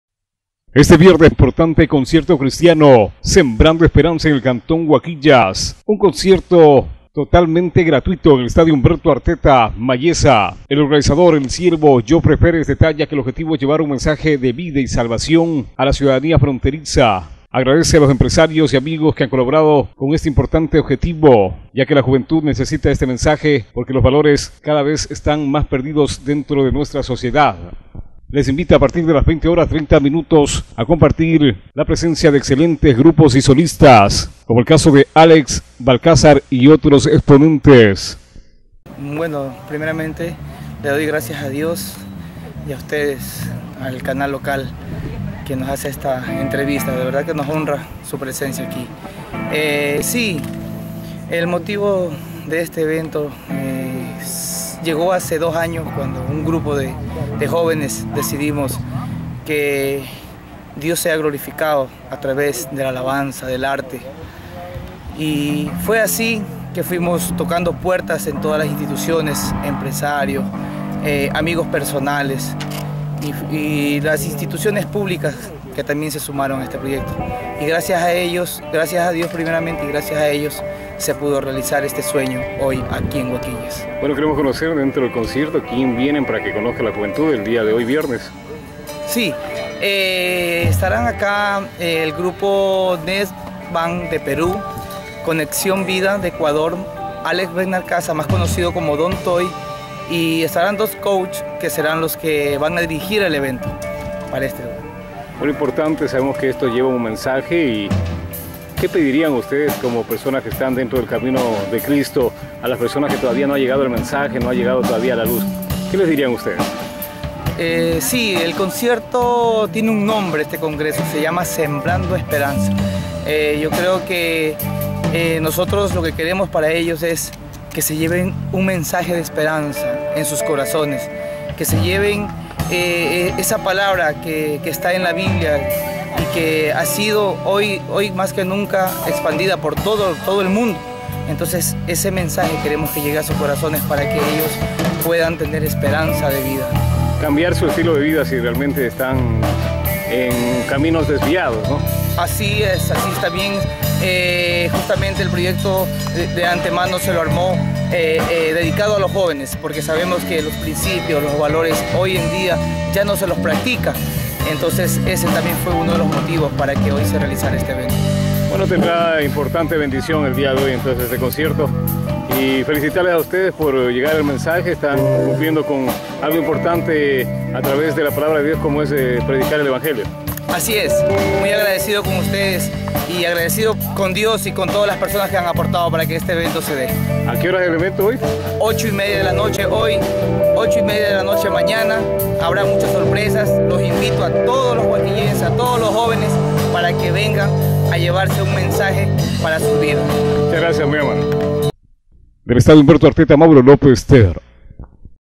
01 AGOSTO CONCIERTO CRISTIANO GRATUITO EN ESTADIO DE HUAQUILLAS
CONCIERTO CRISTIANO ES GRATUITO EN HUAQUILLAS,EL OBJETIVO DEJAR UN MENSAJE DE CAMBIO EN NUESTRAS VIDAS.